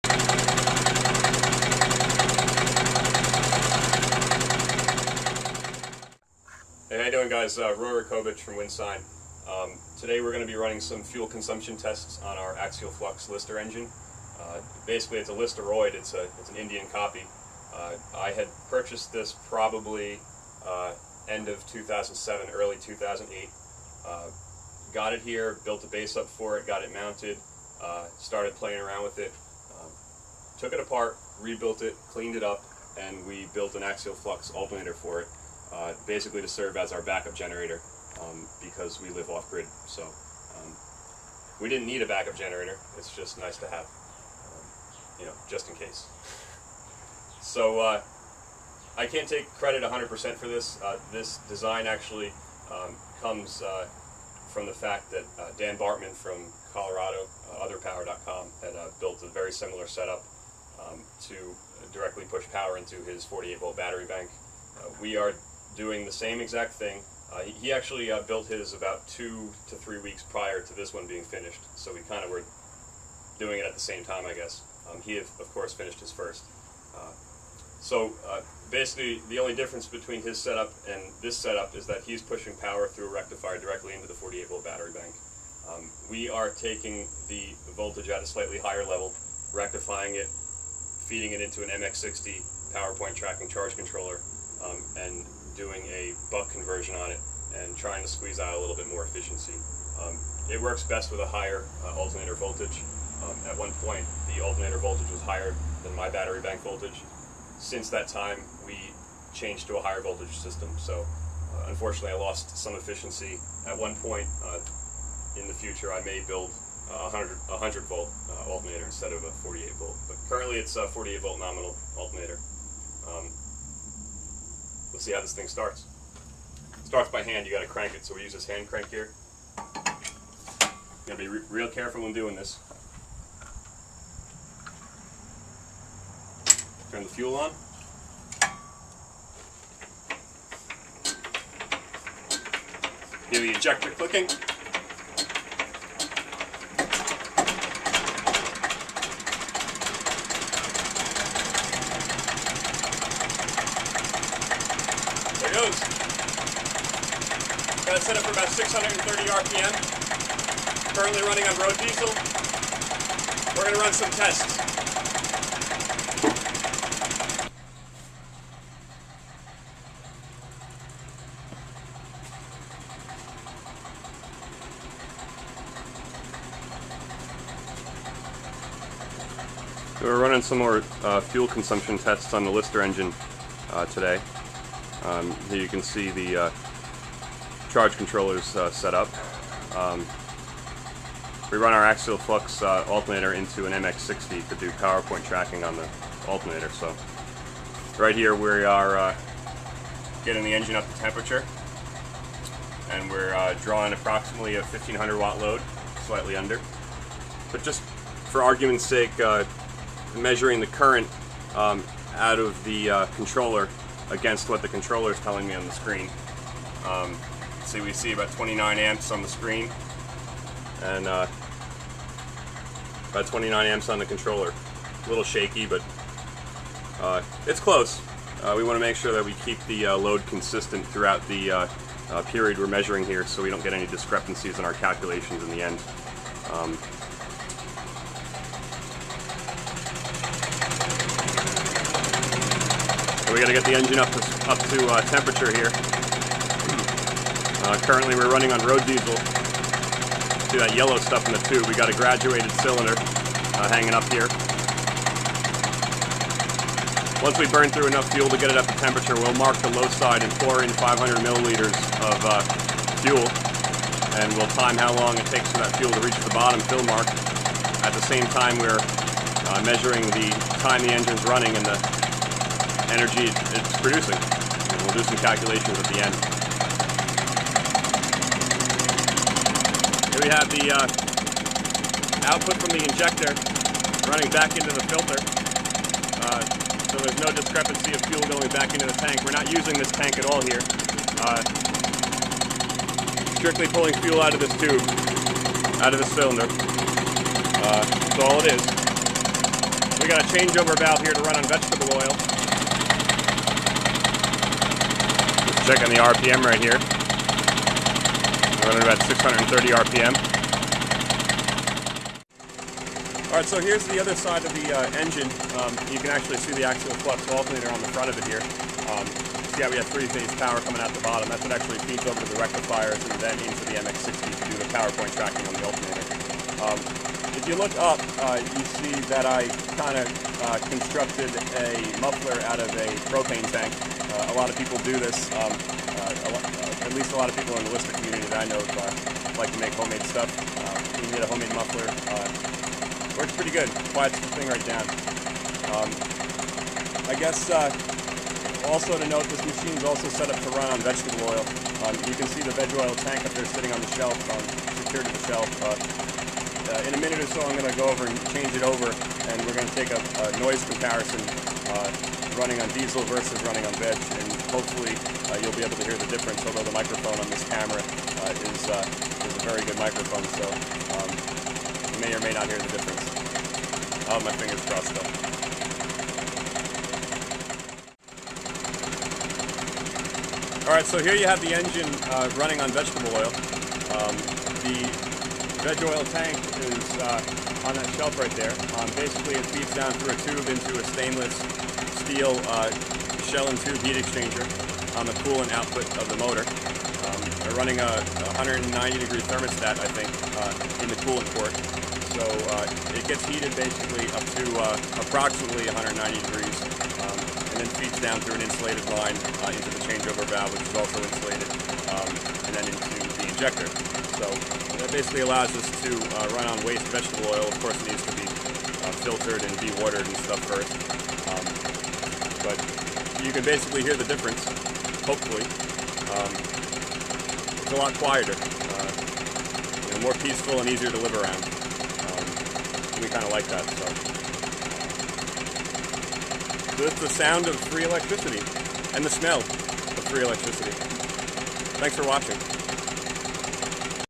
Listeroid with Axial Flux alternator Windsine Inc.
A while ago we built a rather efficient Axial Flux alternator for our listeroid 6/1. We use it basically as an off grid battery charger for our main 48v battery bank (it more or less serves as our back up generator). We finally got around to making a video about it.